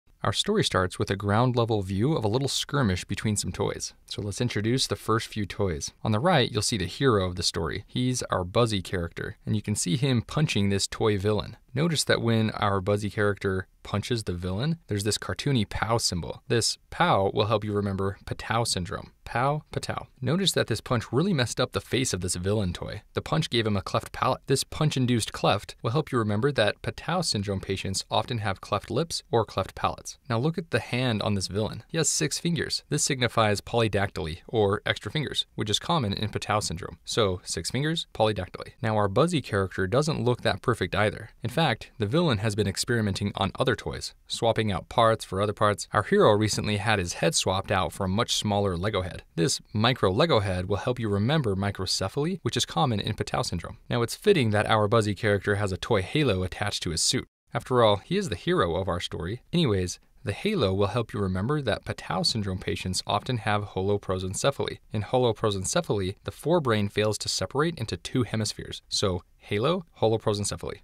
👶 To ace the USMLE, you’ll need more than memorization—especially when it comes to Patau Syndrome.
Physeo’s detailed breakdowns and visual mnemonics allow you to connect chromosomal anomalies to their clinical presentations, giving you both understanding and recall on test day.